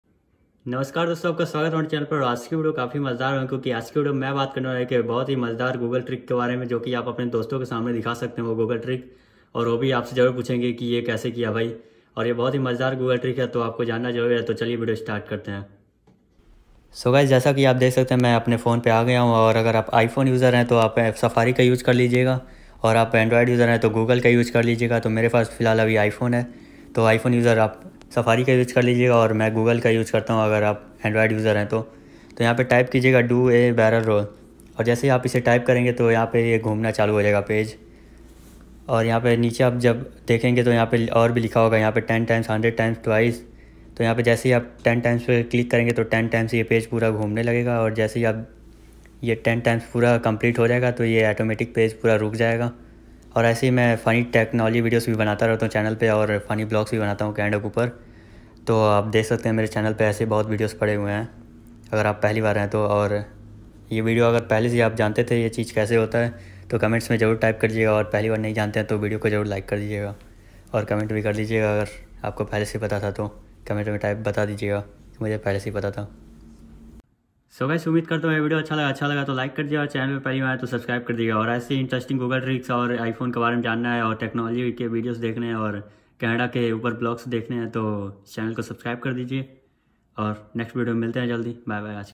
Google’s funny tricks in Hindi
Ees video mai maine aapko Google mai ek funny tricks bataya hai jooke aap aapne dosto ko deka sakte hai

Fun trick in Hindi.

Fun trick on google

And

Facebook DurgeshChaurasiya

            By DurgeshChaurasiya